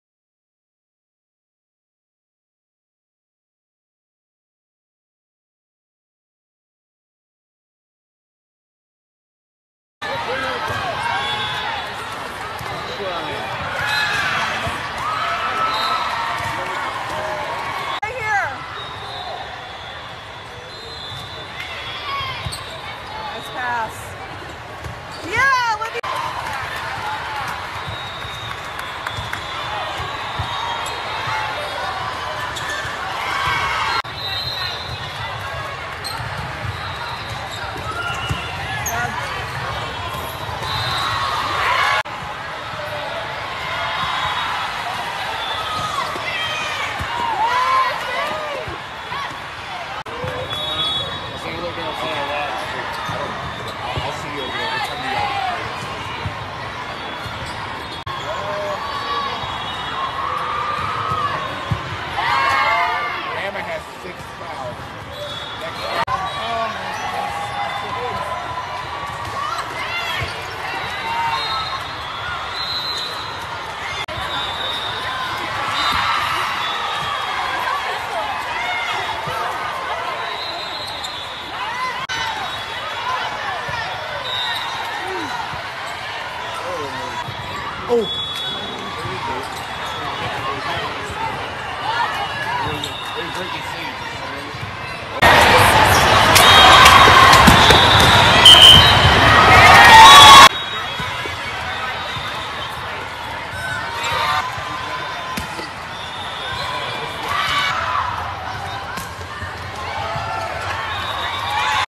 Music City Volleyball Championships 2023 Highlights
Time Stamps:
0:10 Serve Receive
0:42 Digs
1:22 Out of System
1:35 Serves

Faith Ramirez
c/o 2026 Lightridge High School (VA)
Defensive Specialist/Libero
Jersey #18

Club Team: VA Juniors 15 Elite
Club Coach: Dejon Griffin